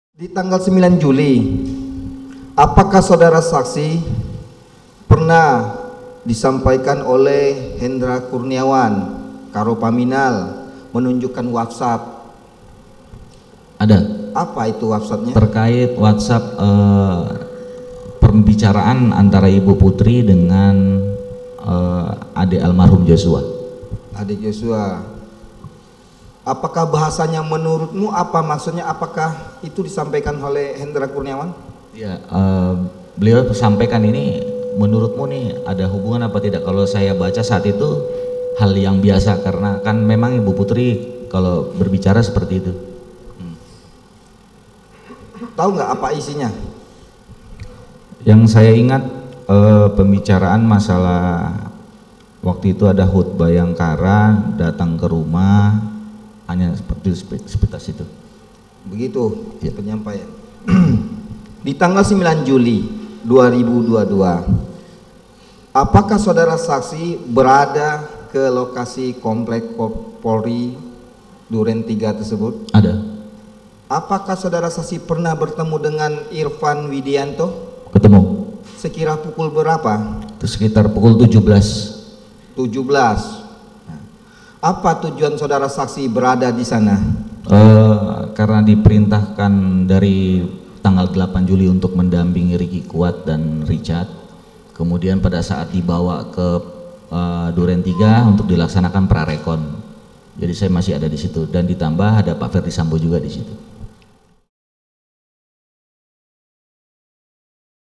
Pengakuan Chuck Putranto Buka WhatsApp Putri Candrawathi dengan Adik Yosua
JAKARTA, KOMPAS.TV - Terdakwa kasus obstruction of justice kasus pembunuhan berencana Nofriansyah Yosua Hutabarat atau Brigadir J, Chuck Putranto mengungkap pesan WhatsApp antara Putri Candrawathi dan adik Yosua.

Hal tersebut diungkap Chuck saat menjadi saksi dalam sidang obstruction of justice untuk terdakwa Arif Rachman Arifin di Pengadilan Negeri Jakarta Selatan, Kamis (12/1/2023).

Baca Juga Ini Alasan Chuck Putranto Tak Lapor ke Ferdy Sambo Saat Ambil DVR CCTV! di

"Kalau saya baca waktu saat itu hal yang biasa, karena kan bu Putri kalau bicara (biasa) seperti itu," kata Chuck.

"Tau enggak apa isinya?" tanya Jaksa.

"Yang saya ingat pembincaraannya masalah HUT Bhayangkara, (adik Yosua diminta) datang ke rumah, sekitar seperti itu," ucap Chuck.

Video editor: Bara Bima Hardika